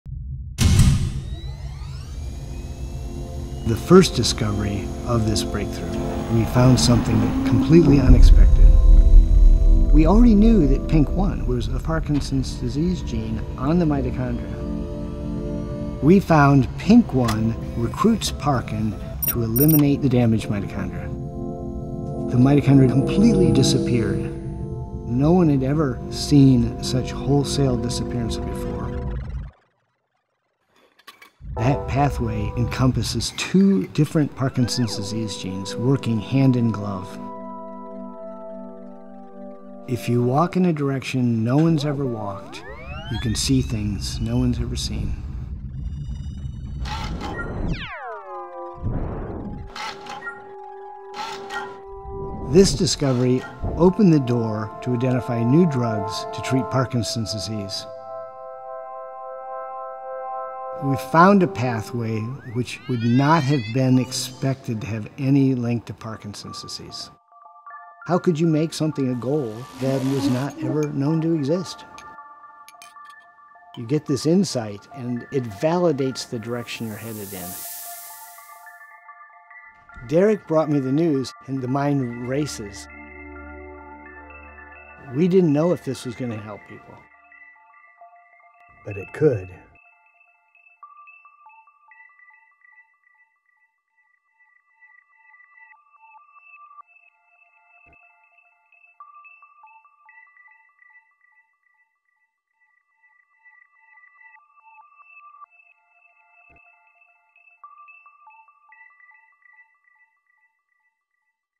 Breakthrough Prize Laureate Short Film: Richard Youle by Grace McNally
Check out this innovative short film, directed by Grace McNally who won the Breakthrough Filmmakers Challenge, which captures a glimpse of a moment of scientific discovery that can transform lives. She zeroes in on the eureka moment when 2021 Breakthrough Prize in Life Sciences winner Richard Youle discovered the function of genes that are mutated in hereditary forms of Parkinson’s disease. Youle figured out that these genes control a cellular pathway that organizes mitophagy - the clearing away of damaged mitochondria. This was significant in that it revealed one of the key links to Parkinson’s at the molecular level, and also showed how autophagy - the clearing and recycling of defective materials in the cell - could be selective: i.e. specific to particular elements of damaged mitochondria.